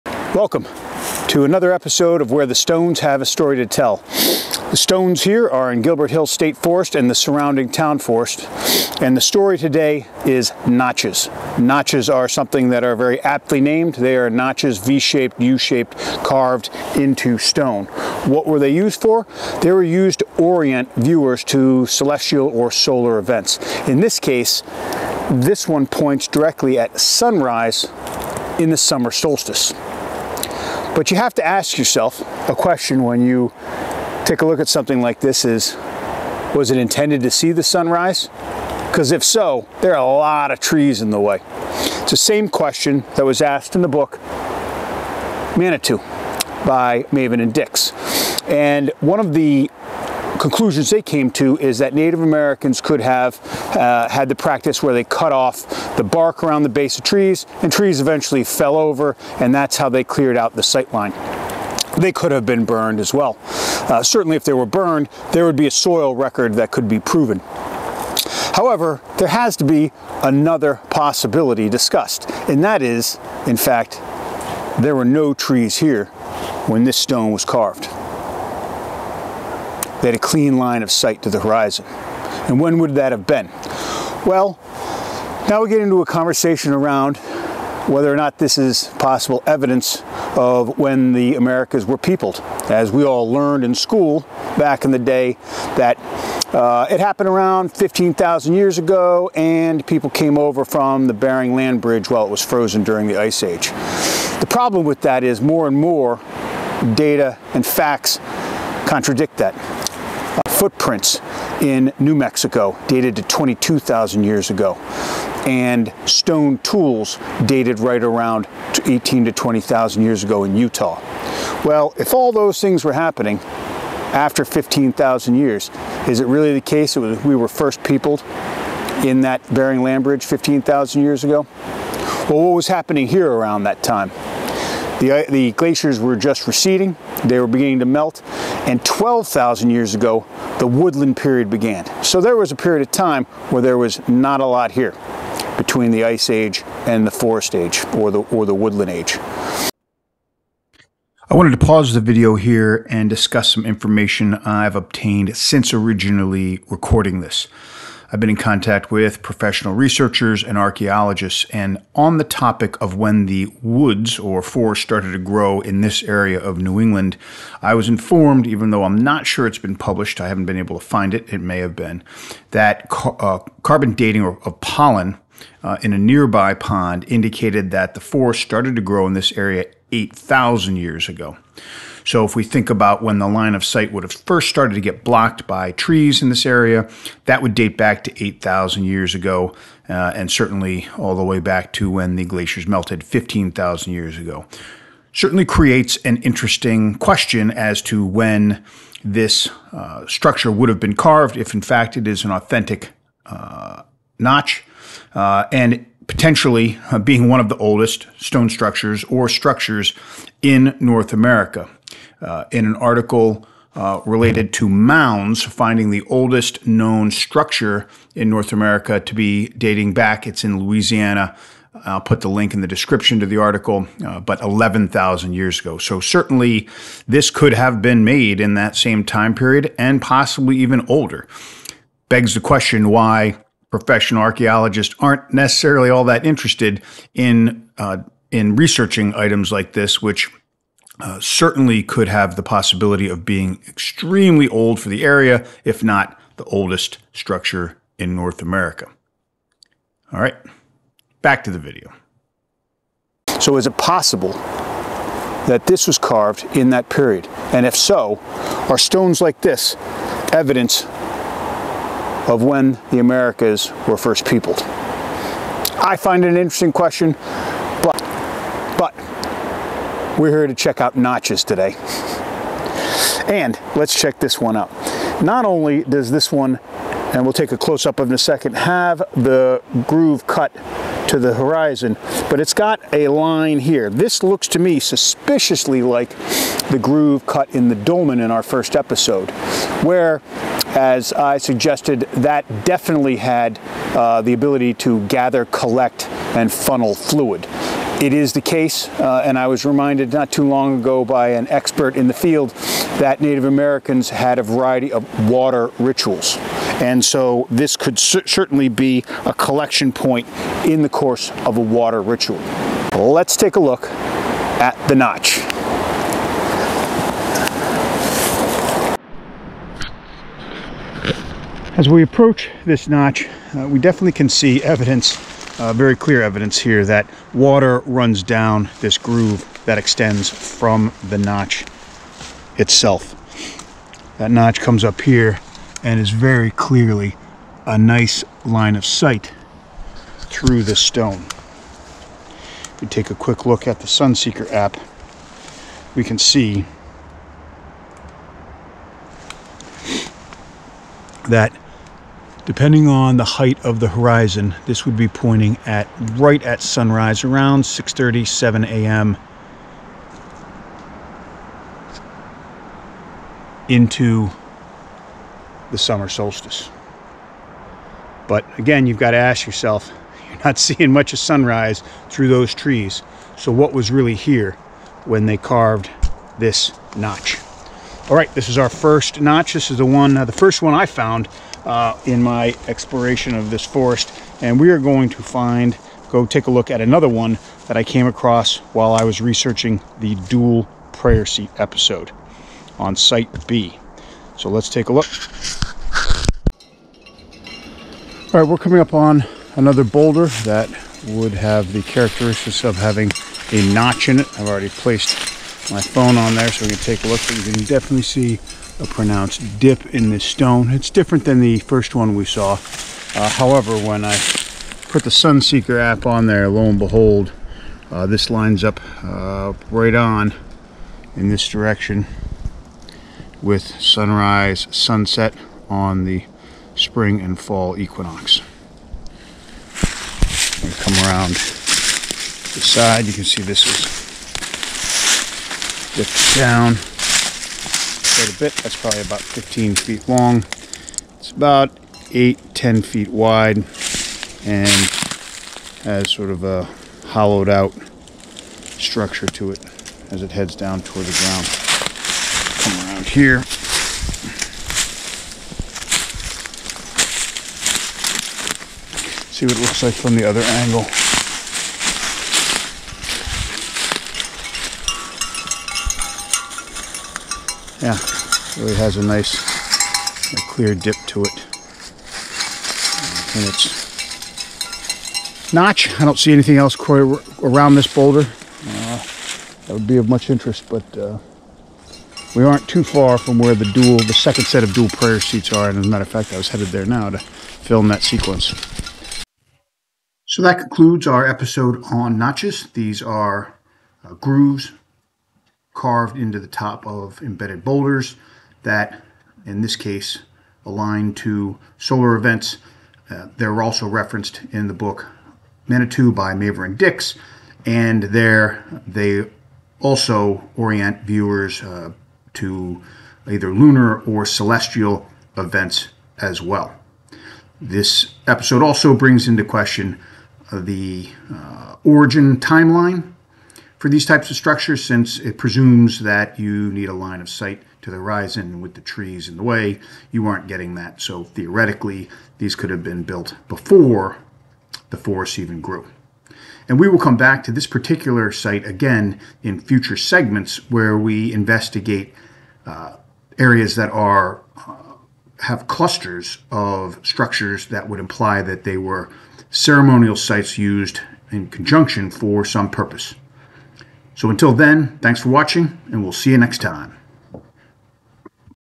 Episode 23- At least 8,000 year old stone structures? Notches Carved in Boulders at Gilbert Hills MA
In episode 23, we demonstrate examples of “notches”.  Notches are believed to be grooves carved into stone to align a viewer to a solar, lunar or celestial event.
It does beg the question, though, that if these structures were meant to view a sunrise or sunset event, how would that have been possible with all of the trees in the way.  It turns out that the trees in this area started growing around 8000 years ago.  So… are structures like notches evidence of very old human activity in this area?  Is it old enough to be relevant to the questions regarding when the Americas were originally peopled?

Notches as Native American Structures: “Manitou”, Mavor & Dix, 1989, p29, 322, 323.